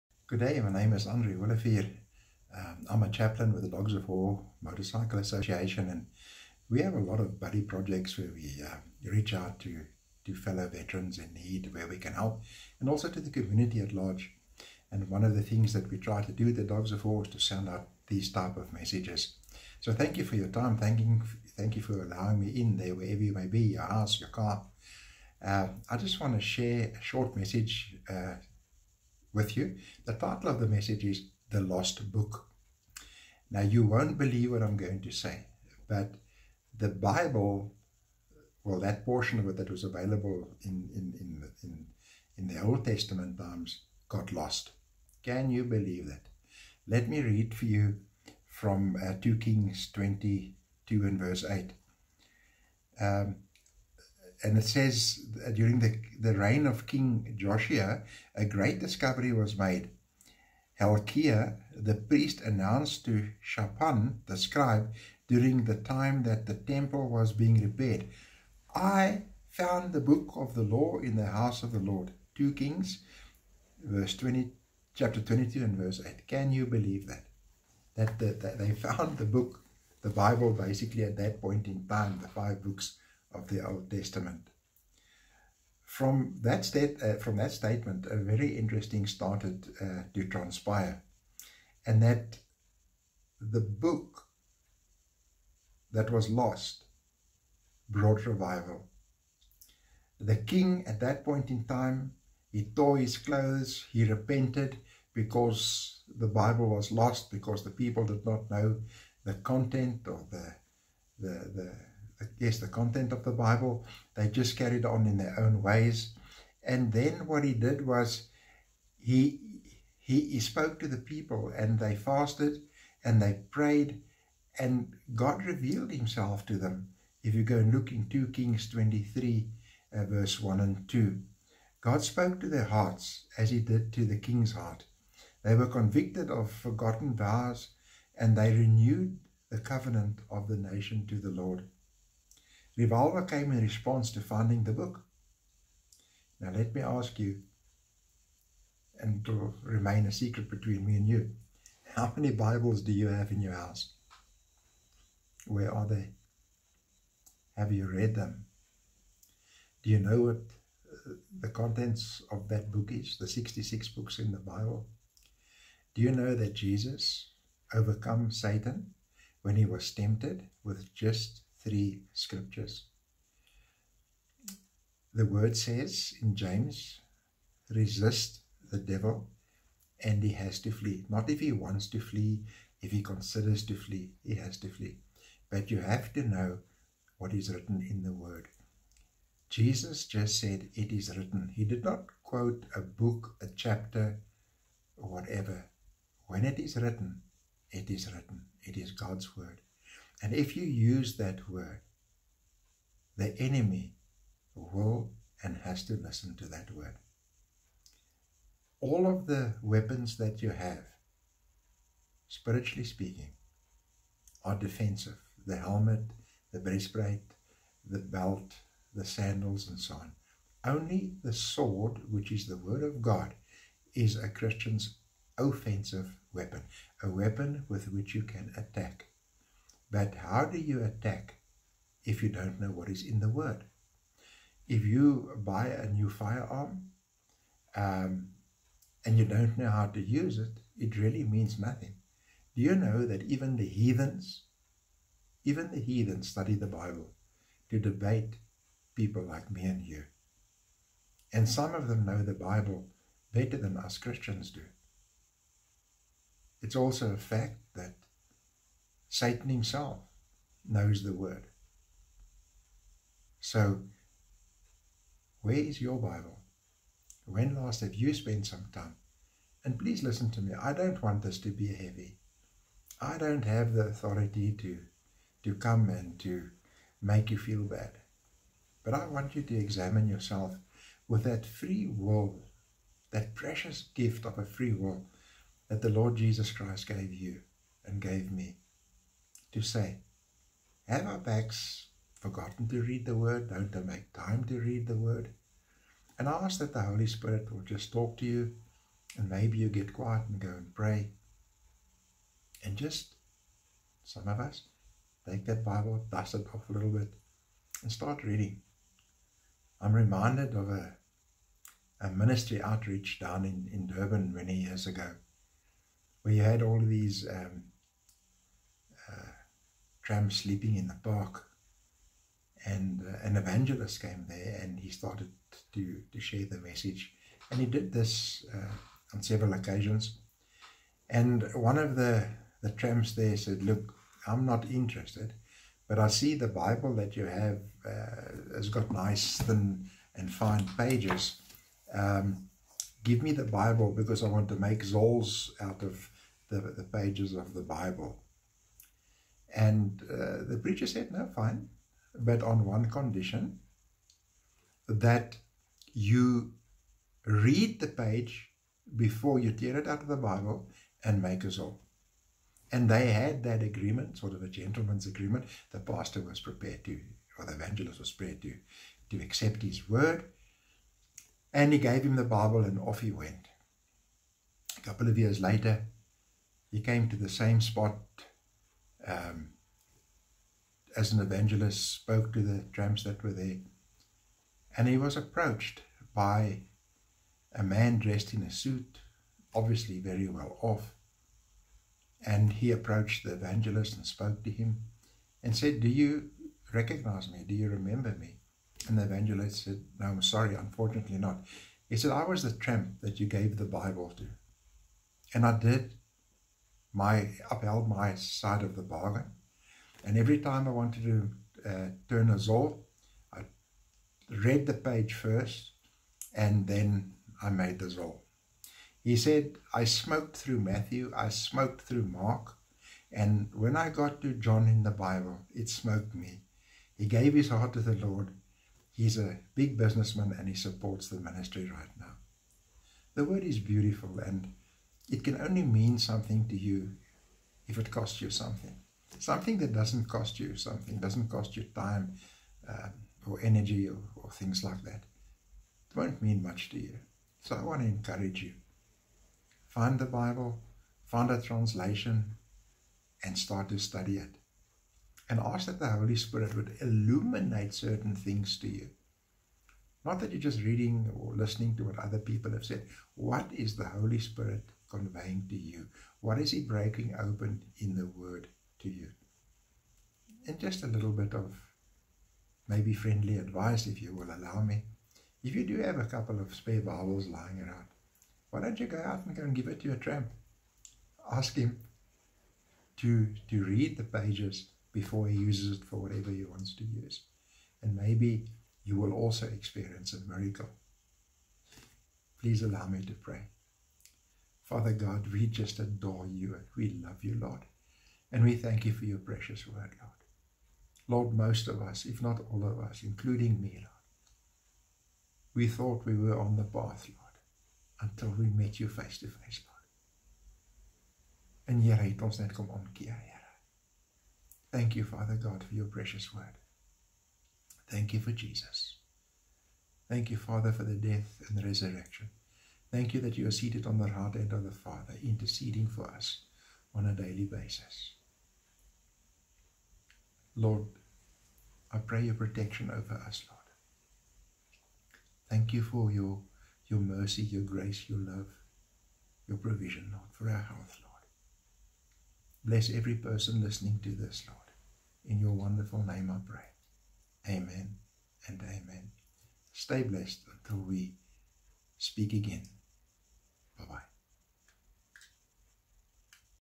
The lost book
Can you believe that the Hebrew scriptures were misplaced and rediscovered?